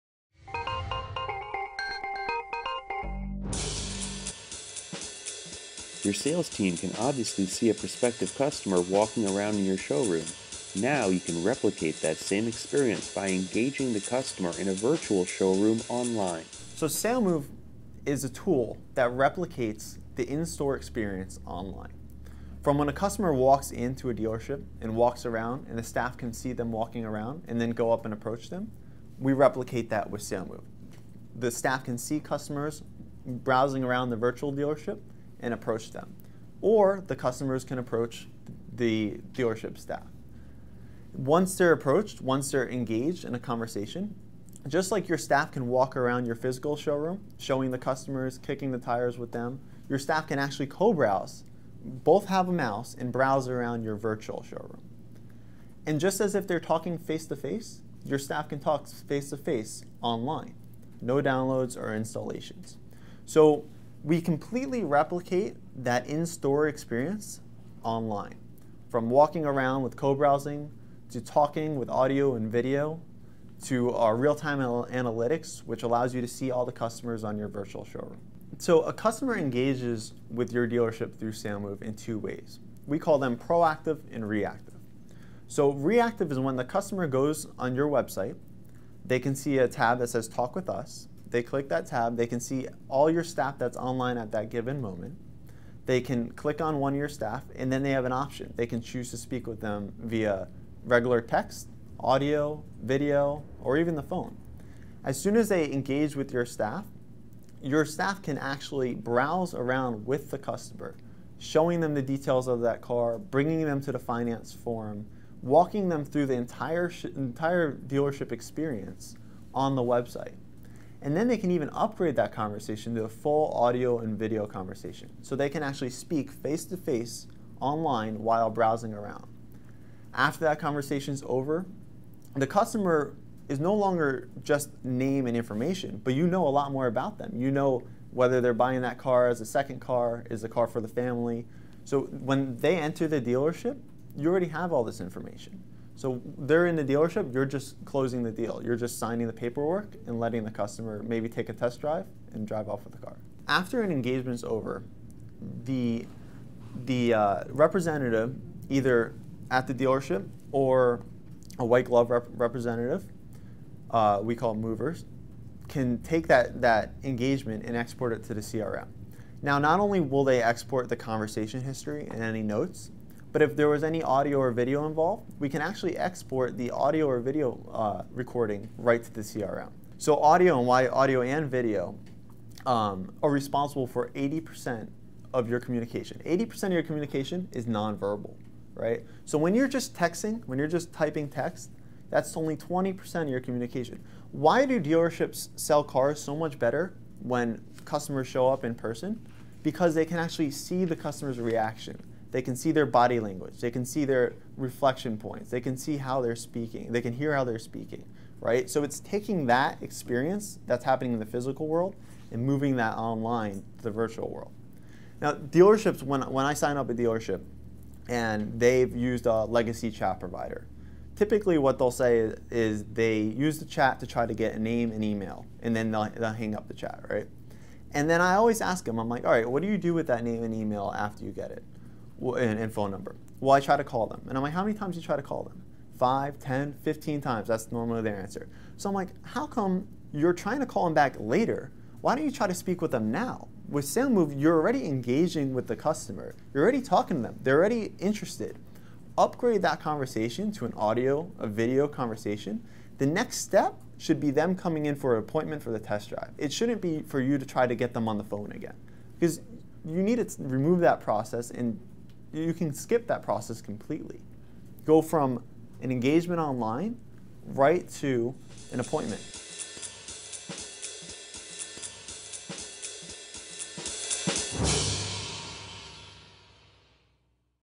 Get Face to Face with Your Customer Online Immediately | Justin DiPietro | Automotive Digest
Just as your salespersons can walk around the showroom in your store, your sales team can now  “browse around” your showroom in a virtual environment with immediate engagement, coaching, and direct one on one assistance in vehicle sales process. See and learn how you can capitalize on the fact 80% of your communication is non-verbal.

Justin DiPietro, Co-Founder & COO, SaleMove.